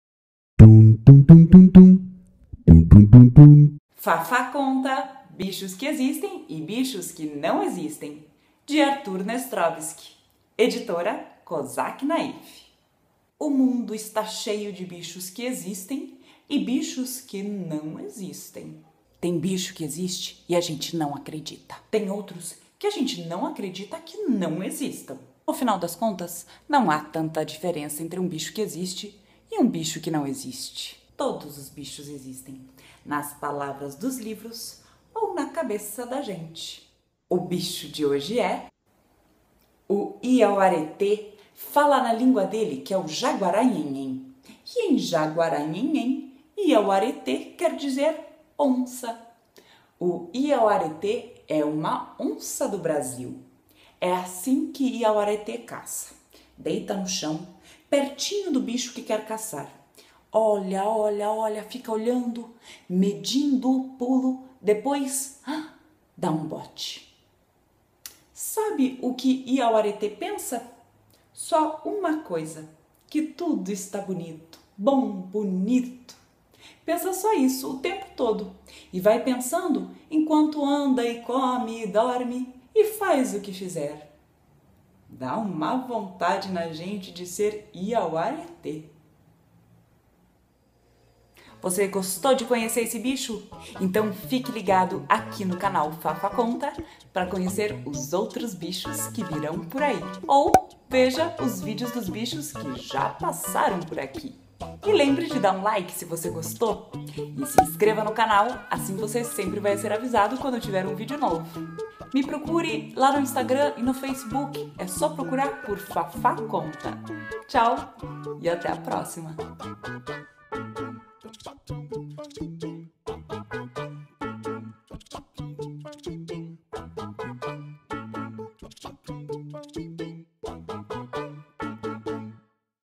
IAUARETÊ - Série "Bichos que existem e bichos que não existem" por Fafá conta
Livro: Bichos que existem e bichos que não existem de Arthur Nestrovski. Editora Cosac Naify

Acompanhe a série: toda segunda e sexta tem novos vídeos aqui no canal Fafá Conta!